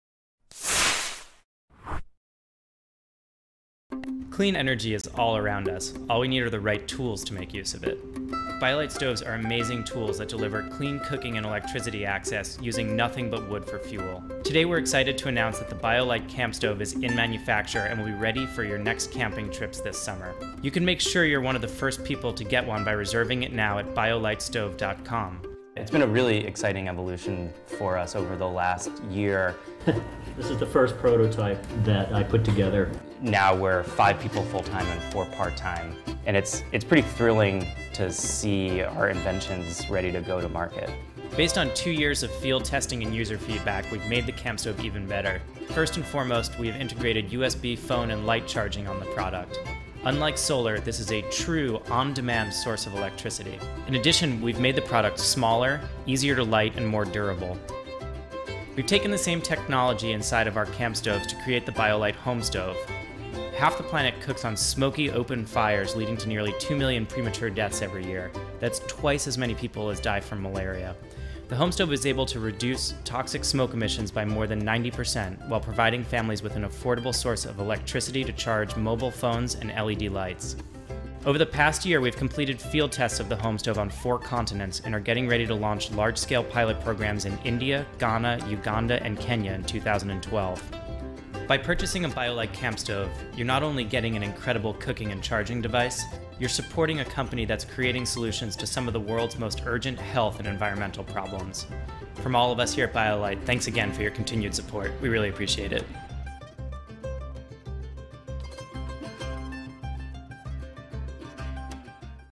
Dùng bếp củi để... sạc pin iPhone
Đa số những loại bếp củi phổ biến trên thế giới đều sử dụng nhiều gỗ hoặc các chất cháy vào lúc mồi lửa nhưng sau đó, đa số năng lượng phát ra từ bếp củi đều chưa được tận dụng một cách hiệu quả dẫn đến tình trạng lãng phí và tạo ra khí độc gây hại cho môi trường và sức khỏe con người. Giống như Inyenyeri, một sản phẩm xuất hiện tại New York với tên BioLite là một loại bếp củi chỉ sử dụng 1/2 lượng gỗ hoặc các chất cháy, gần như không thải ra khói và đặc biệt hơn, BioLite tận dụng năng lượng thừa để tạo ra điện cho các mục đích khác nhau.

Được tạo ra phù hợp với những chuyến đi cắm trại, công nghệ của BioLite giúp chuyển đổi nhiệt năng thành điện năng một cách triệt để và hoàn toàn tự động. Bằng cách tối ưu hóa việc đốt, BioLite chỉ sử dụng 1/2 lượng củi so với các loại lò khác và có thể giảm đến 90% khói. Với nguồn điện tạo ra, người dùng có thể cắm sạc iPhone qua cổng USB nối vào lò, dùng cấp nguồn điện cho đèn LED hoặc các thiết bị GPS... Bạn có thể xem video trên đây để thấy rõ hơn công dụng của BioLite.

Được bán ra với giá 129$, BioLite's CampStove (cho các chuyến dã ngoại) sẽ có mặt vào mùa hè này, phiên bản HomeStove (cho gia đình) cũng được phát hành đồng thời và phân phối rộng khắp đến Ấn Độ, Ghana, Uganda và Kenya. BioLite được đánh giá là một mô hình doanh nghiệp xã hội vô cùng sáng tạo, giải quyết vấn đề năng lượng bằng hoạt động kinh doanh của mình.